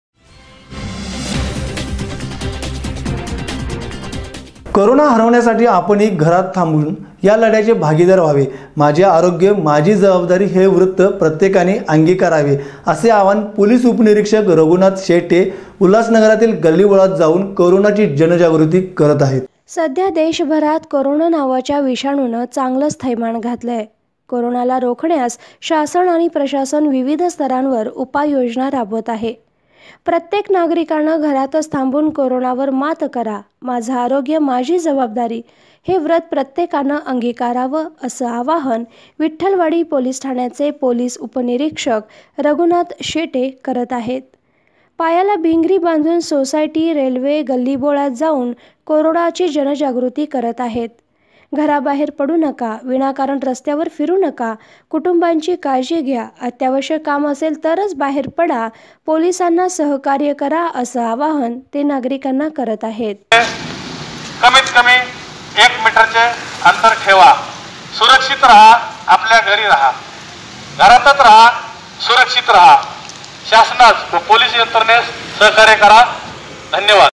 'माझे आरोग्य, माझी जबाबदारी' हे व्रत प्रत्येकाने अंगीकारावे
'माझे आरोग्य, माझी जबाबदारी' हे व्रत प्रत्येकाने अंगीकारावे - पोलीस उपनिरीक्षक रघुनाथ शेटे उल्हासनगरातील गल्लीबोळात जाऊन आवाहन.@